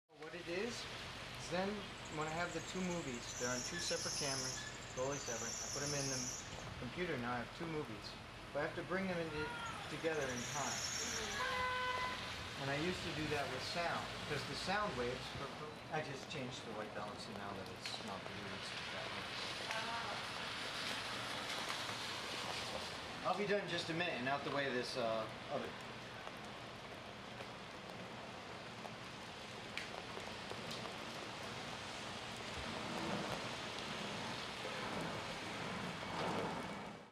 3D in the Rain
Stereoscopic 3D video of 7th Avenue, Brooklyn